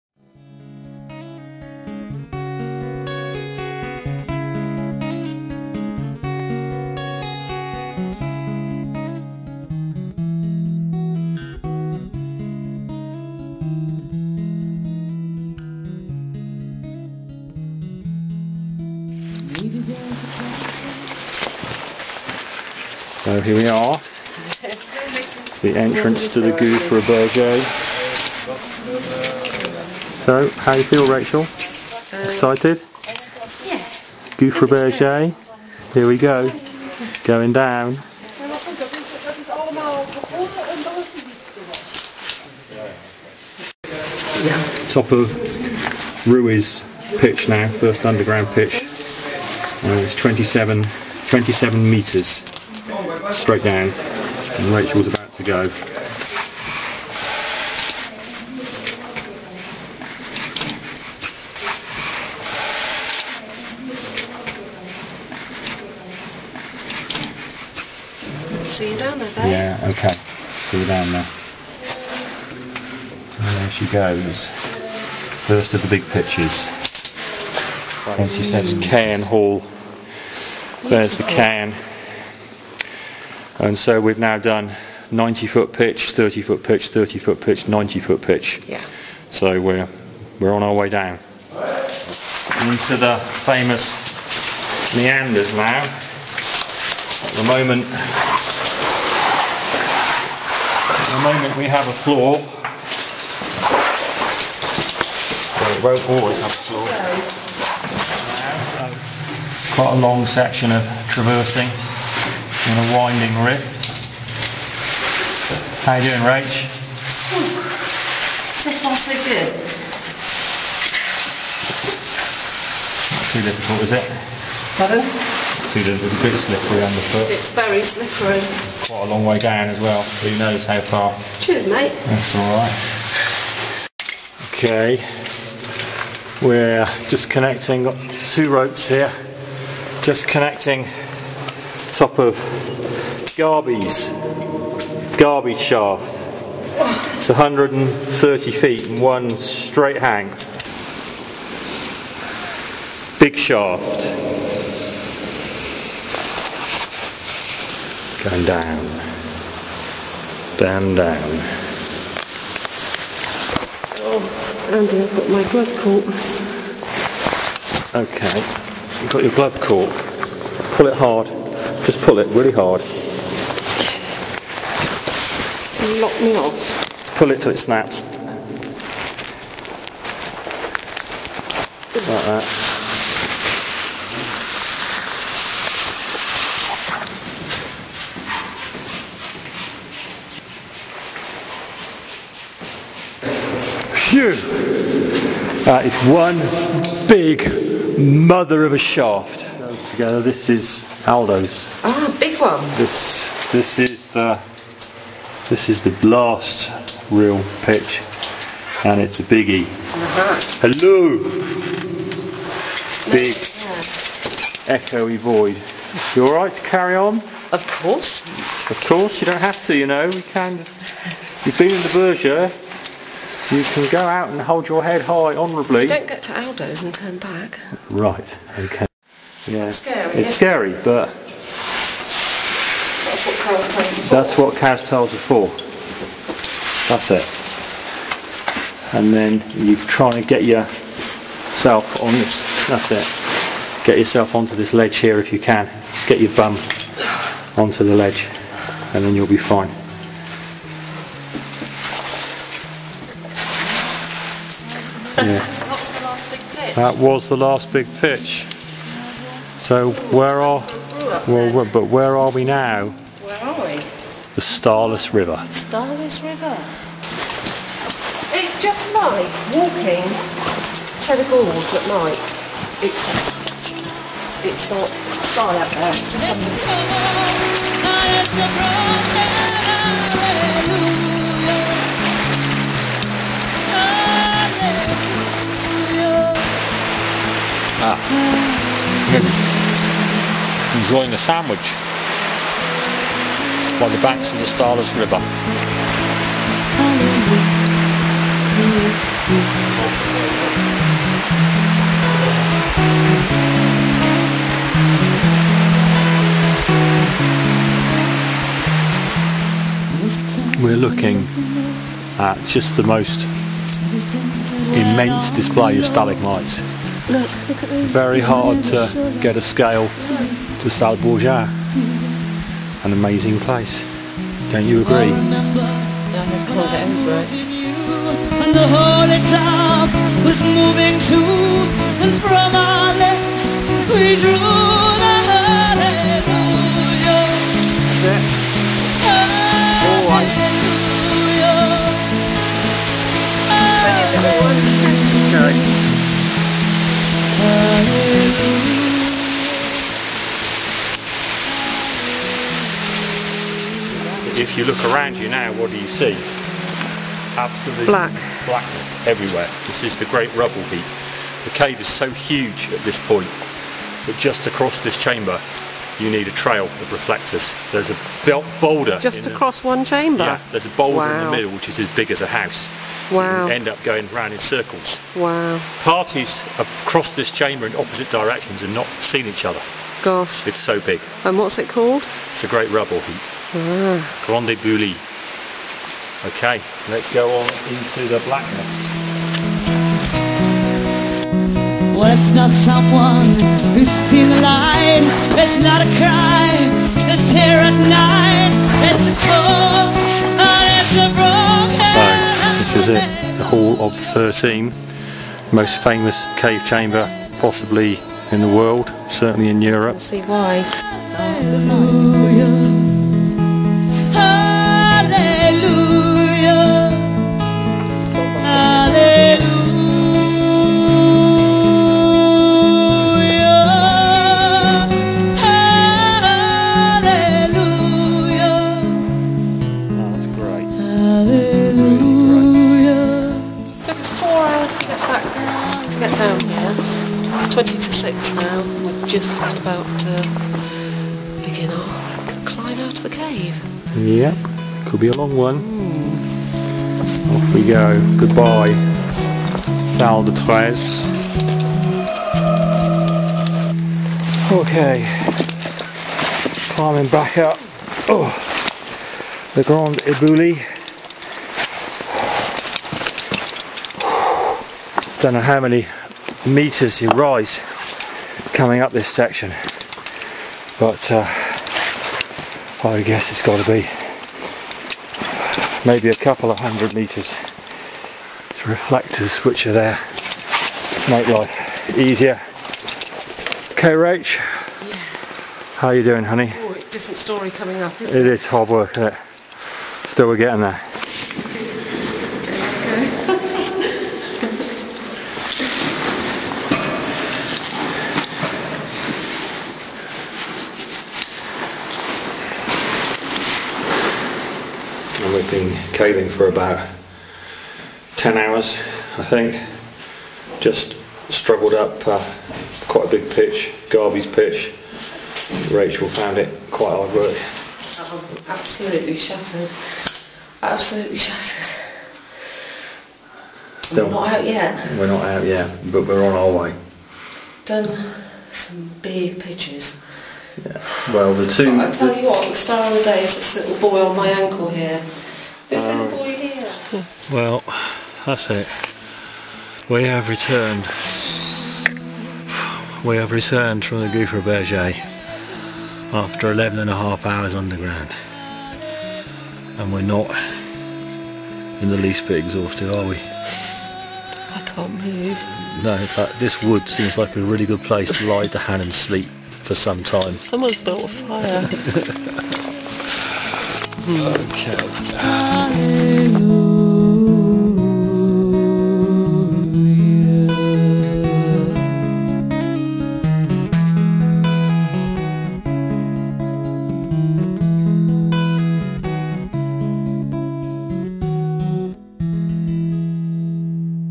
500 metres down.wmv
Andy and Rachel Sparrow descend the renowned Gouffre Berger to admire the famous 'Hall Of The Thirteen'.   A 10 minute film.